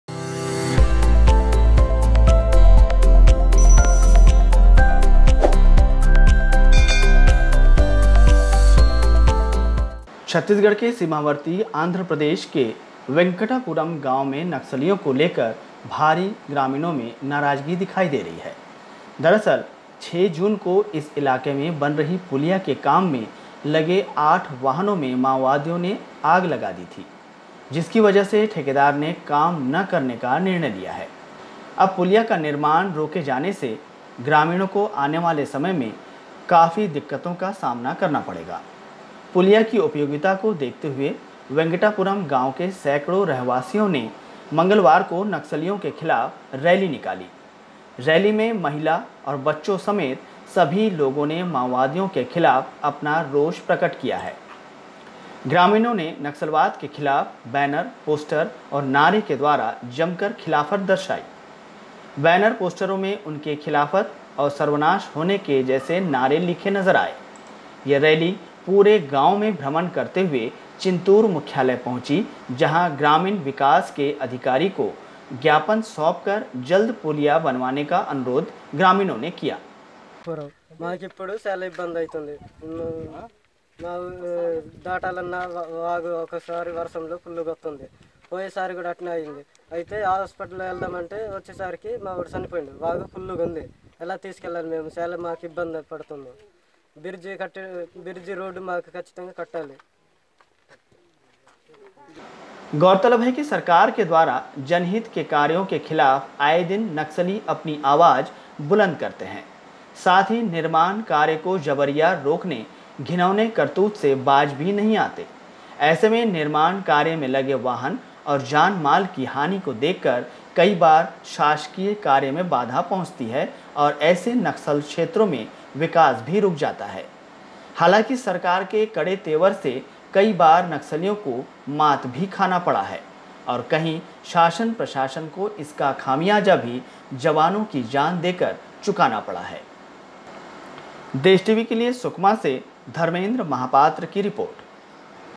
नक्सलियों के खिलाफ ग्रामीणों ने जताई नाराजगी|Protest Against Naxalites| CG-Andhra Border|CG News
छग-आंध्र सीमा पर नक्सलियों के खिलाफ नाराजगी, ग्रामीणों ने नक्सल खात्मे पर बैनर पोस्टर के साथ निकाली रैली